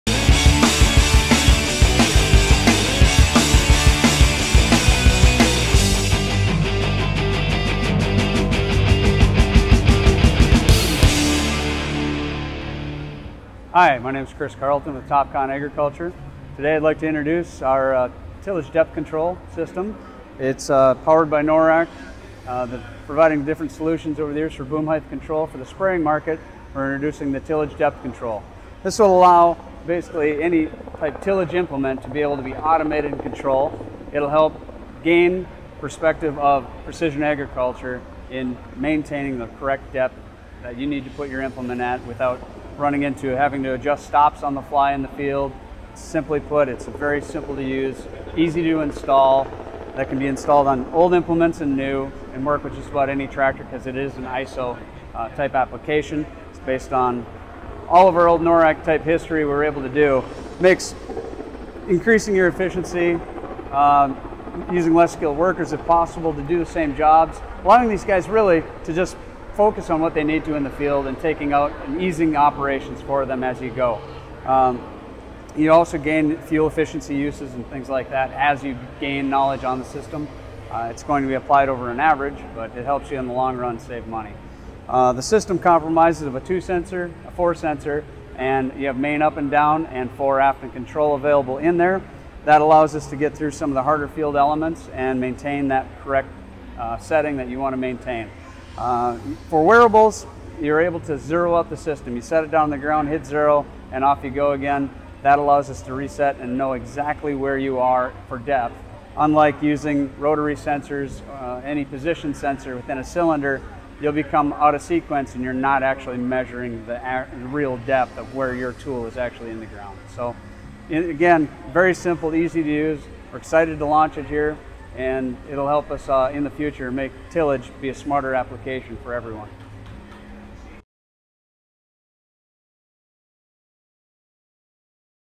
Topcon Agriculture’s New Tillage Depth Control System Powered by Norac
Chris Carleton, OEM Account Manager for Topcon Agriculture introduces the new tillage depth control system. Allowing a tillage implement to be automated and controlled, the new system, powered by Norac, the depth control system will allow growers to maintain the correct depth without having to make adjustments on the fly while in the field.